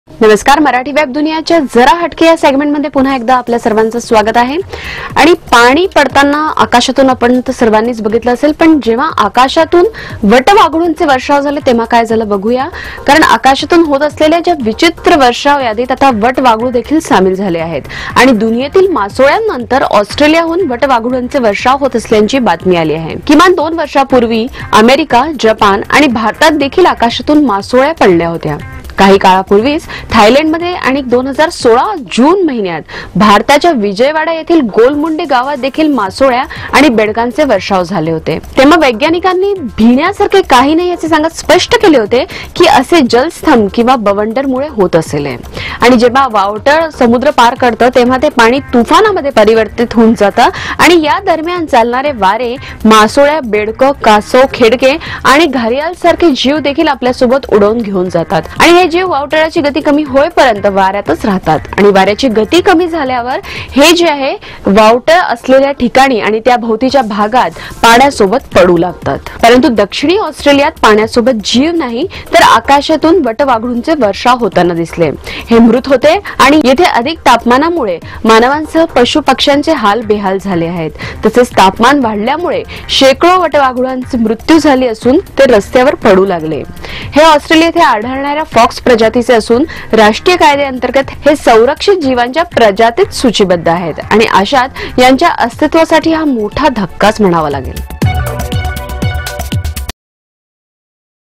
आकाशातून वटवाघुळांचे वर्षाव Bats Rain
आकाशातून होत असलेल्या विचित्र वर्षावच्या यादीत वटवाघूळदेखील सामील झाले आहे.